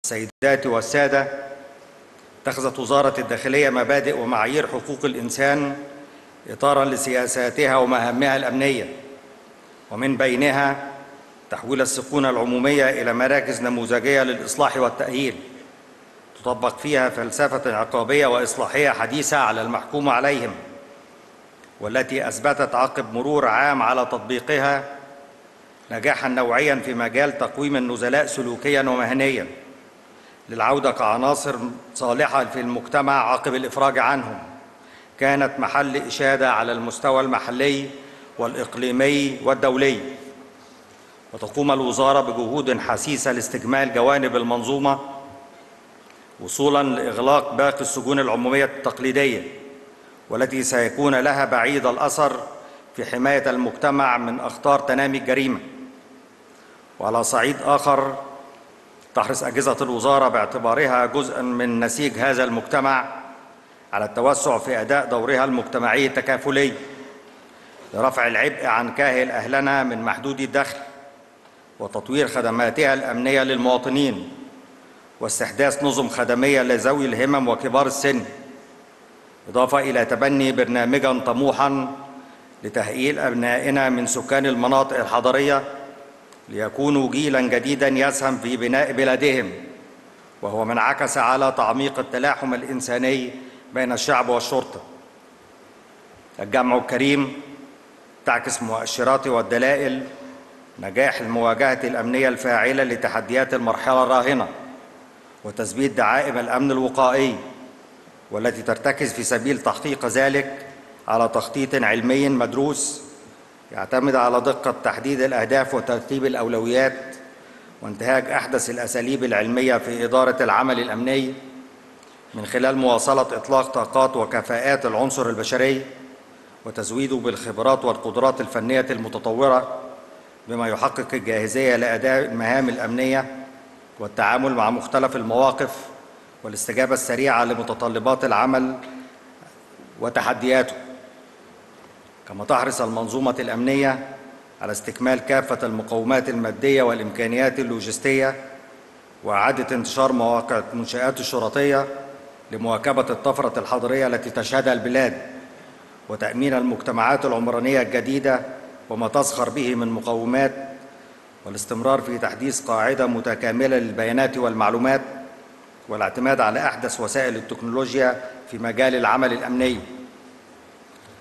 وزير الداخلية: إتخذت وزارة الداخلية مبادئ ومعايير حقوق الإنسان إطارًا لسياستها ومهامها الأمنية
أخبار أون
نقل فعاليات احتفالية وزارة الداخلية بعيد الشرطة الـ71 بحضور الرئيس السيسي في أكاديمية الشرطة والسيسي يضع إكليلًا من الزهور على النصب التذكاري لشهداء الشرطة ومدير إدارة المعلومات بقطاع الأمن يوضح التطور الكبير لوزارة الداخلية للتسهيل على المواطن ورائد هند محمد علام توضح أهمية تكريم الرئيس السيسي لها في يوم إحتفالية عيد الشرطة وكلمة حرم اللواءعبد الكريم درويش أحد ضباط معركة الإسماعيلية وعرض فيلم بطولة القدير أحمد يدير يحكي عن بطولات الشرطة المصرية في عيدها الـ 71